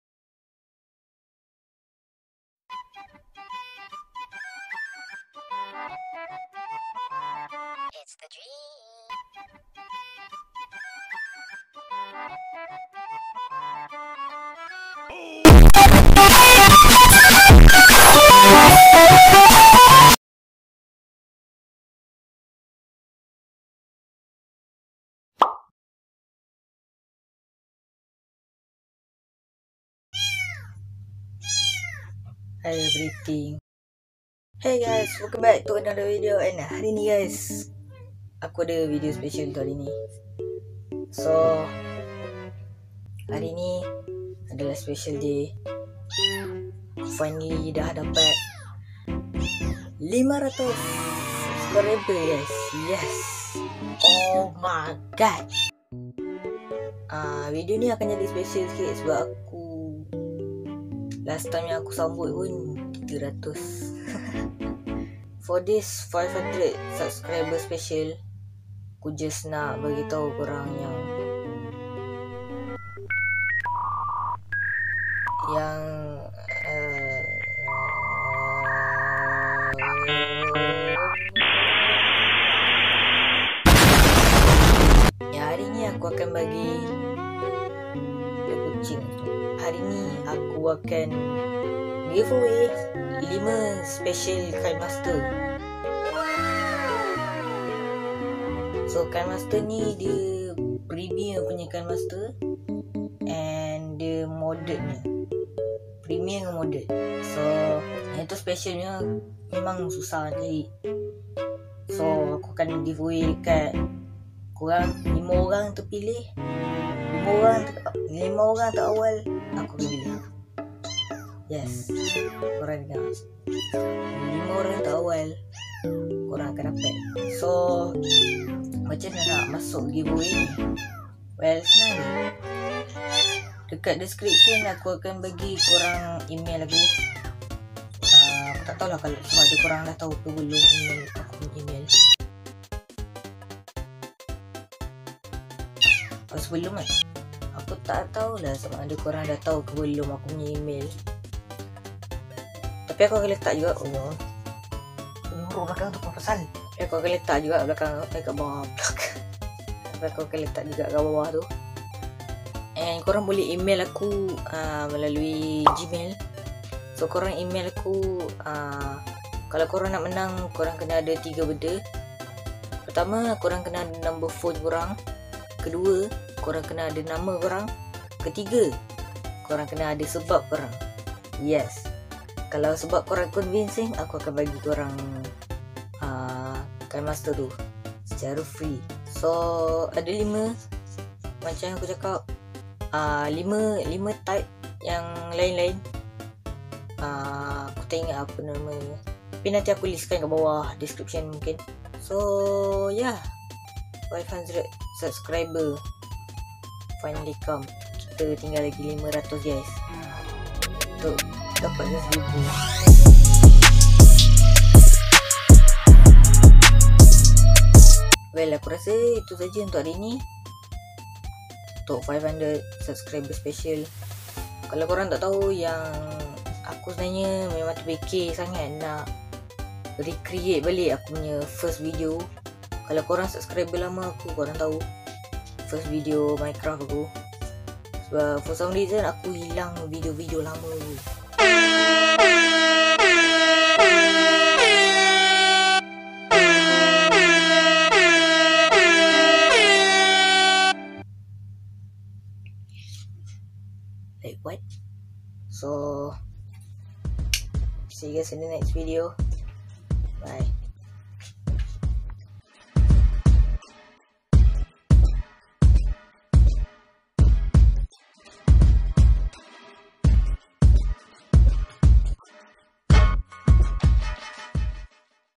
My First Giveaway!!! | 500 Subscribers Special (Instruction kat description.
Hey guys!!! Ada lima yang aku akan bagi kepada yang terpantas.

1. Kinemaster Pro Mod
2. Kinemaster Legend
3. Kinemaster Diamond
4. Kinemaster Ruby
5. Kinemaster Gold

Korang boleh masuk giveaway dengan email aku dan ikut step step ni:
1. Tulis email/nombor phone korang
2. Tulis nama korang
3. Tulis sebab kenapa nak Kinemaster ni

Email aku kalau nak masuk. Good luck!

Follow Instagram aku: FarisFG
Follow twitter aku: DanGamingOnline